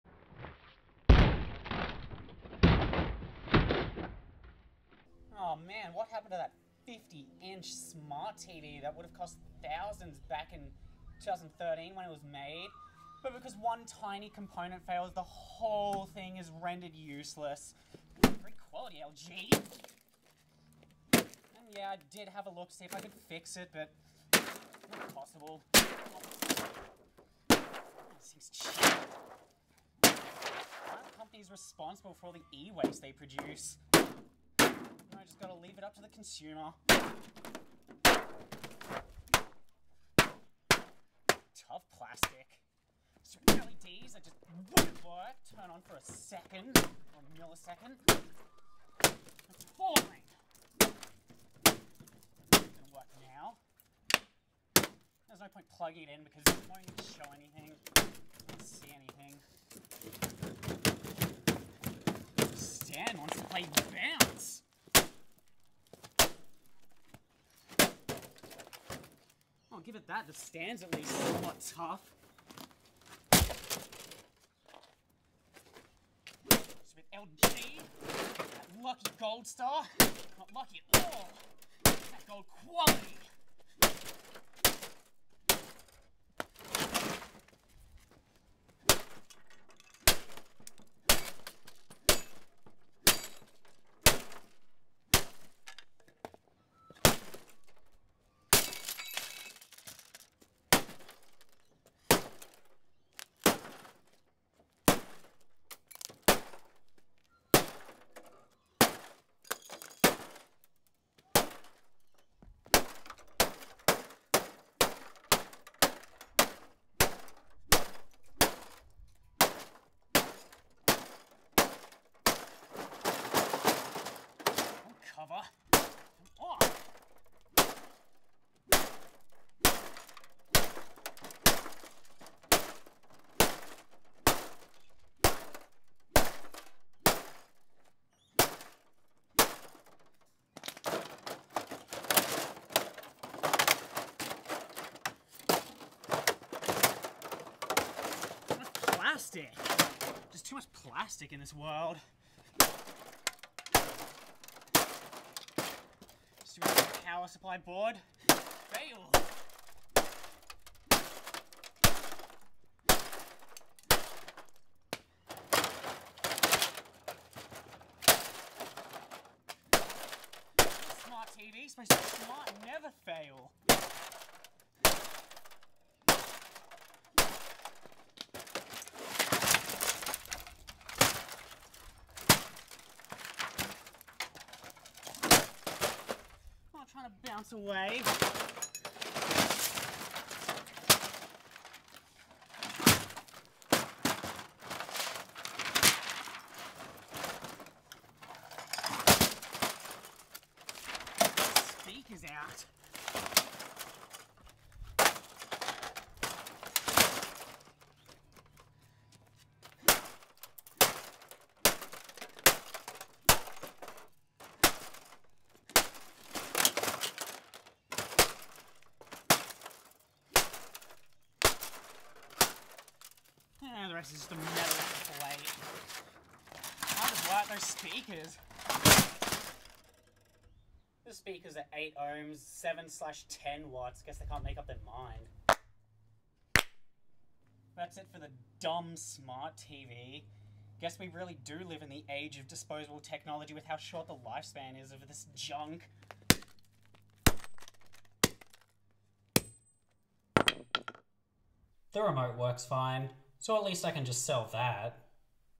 Faulty LG 50" LED Smart TV Destruction
Faulty 50" LED smart TV wasn't smart at all!

Model: LG 50LA6230-TB 50" LED smart TV. Made in June, 2013.